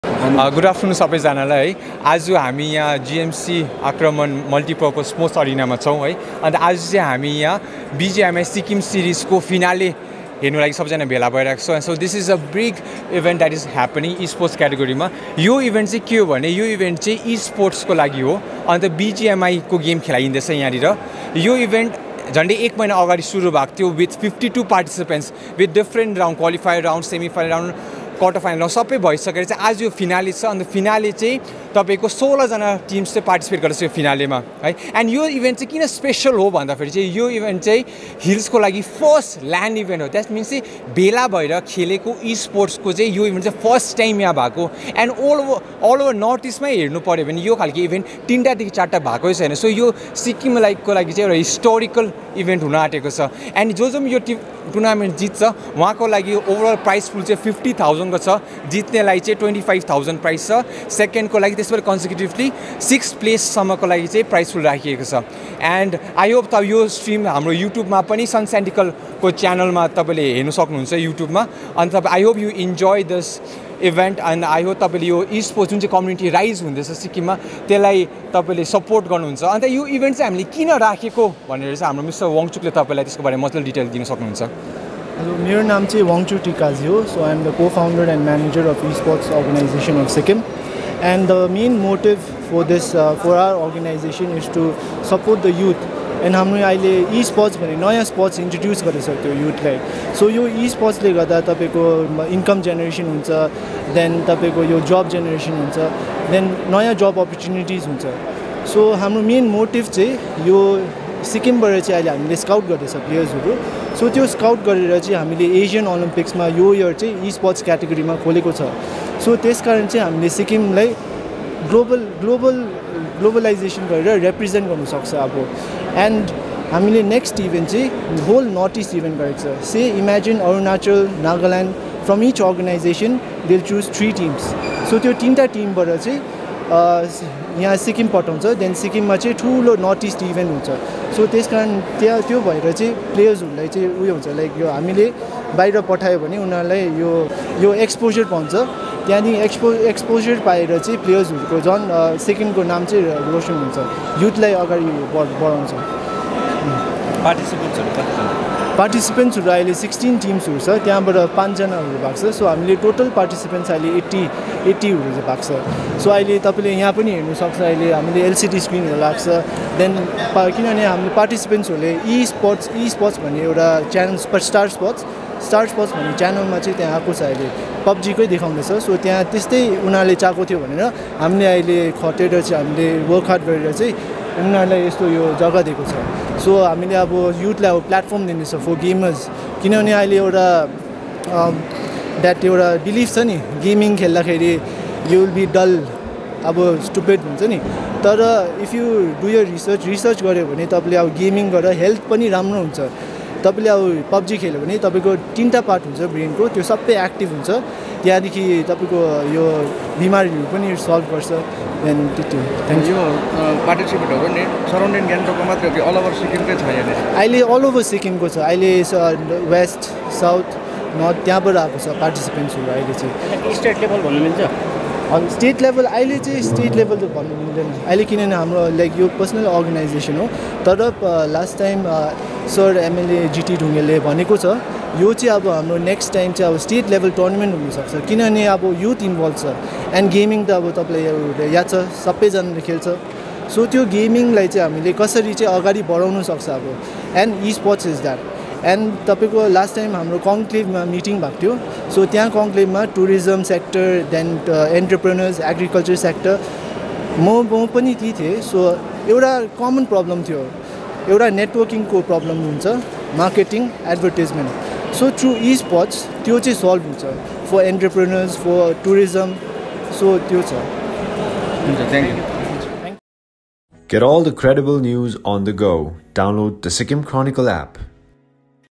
Esports organization of Sikkim organizes BGMI Sikkim Series S1 on July 2 at GMC Sports Arena.
This is the first BGMI LAN event held in Sikkim.